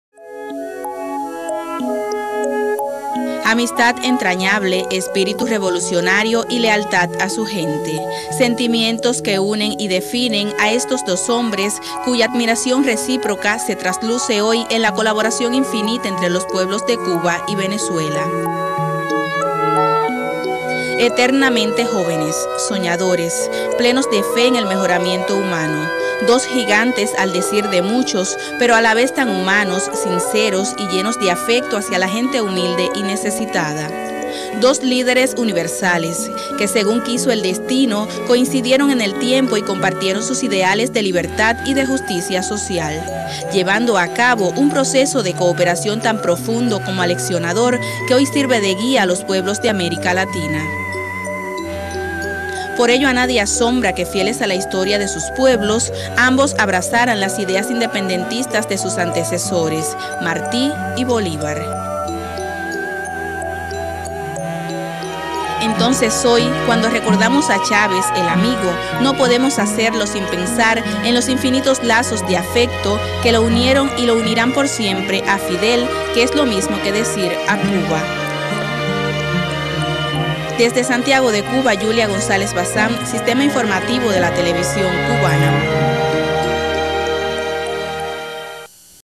Amistad entrañable
Hugo Chávez llega a Cuba, por primera vez, el 14 de diciembre del año 1994. Fue Fidel quien lo recibió en el aeropuerto internacional "José Martí". Desde entonces una gran amistad los unió y la admiración y respeto fue recíproco.